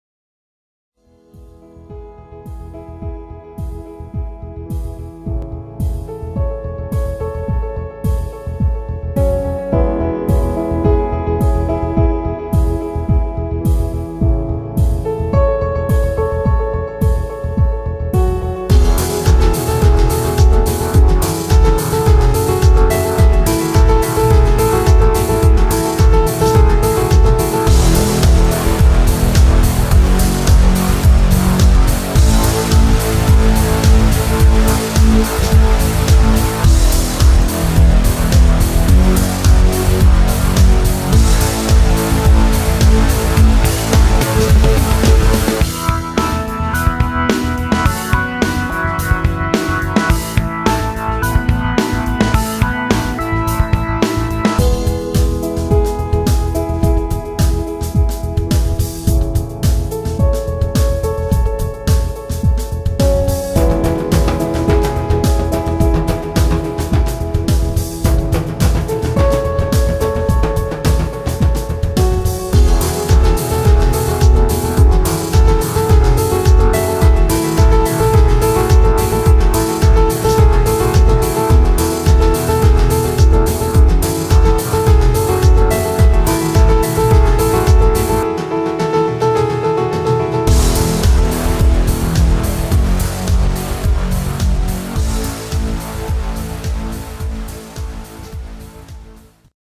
Who is Paul J. Clark? (Album Trailer)
Unsigned independent artist Paul J. Clark released his debut full length album.  This is his trailer.